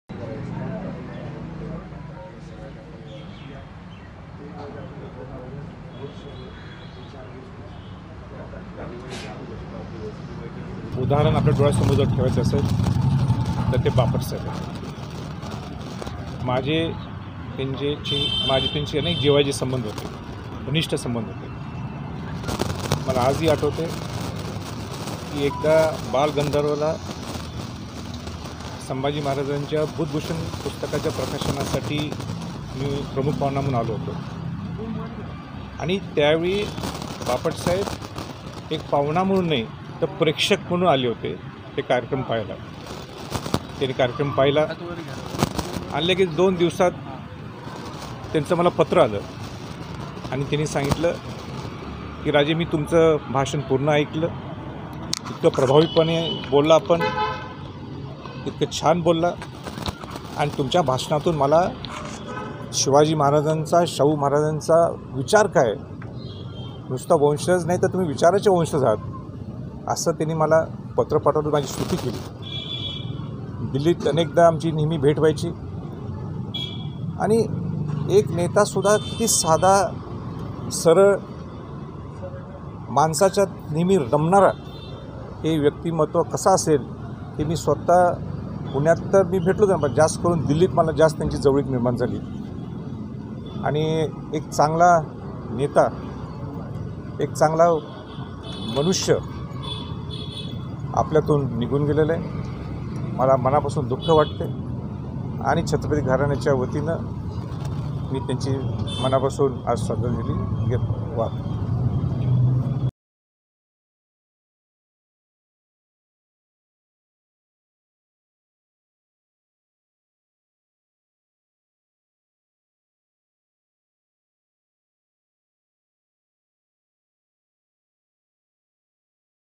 Sambhaji Raje Bhosale : बापटांचं पत्र आलं... संभाजीराजेंनी सांगितलं आठवण | LetsUpp Marathi
भाजपचे खासदार गिरिश बापटांच्या निधनानंतर छत्रपती संभाजीराजे भोसलेंनी बापट कुटुंबीयांची भेट घेतली. दरम्यान संभाजीराजेंनी अनेक आठवणींना उजाळा दिला.